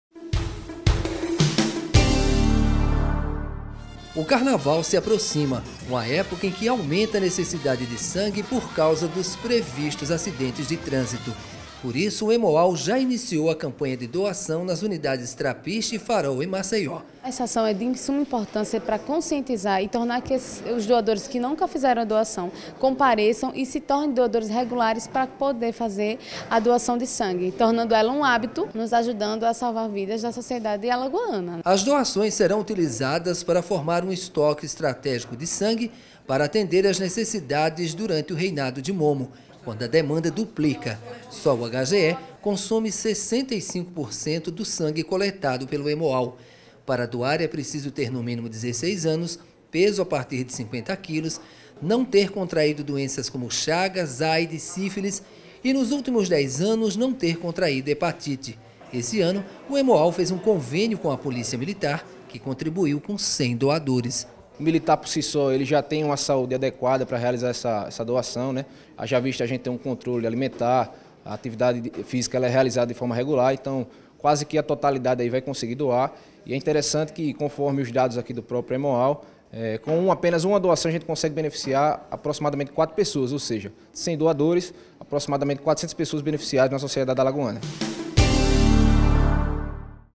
E em Dia - Hemoal Carnaval
O Hemoal já iniciou a campanha de doação de sangue nas unidades do Trapiche e Farol em Maceió, visando manter um estoque de sangue para o período de carnaval.
Para doar sangue, basta ter no mínimo 16 anos, peso a partir de 50kg e não ter contraído doenças como Chagas, Aids, Sífilis e, nos últimos 10 anos, não ter contraído Hepatite.
Venha participar dessa campanha!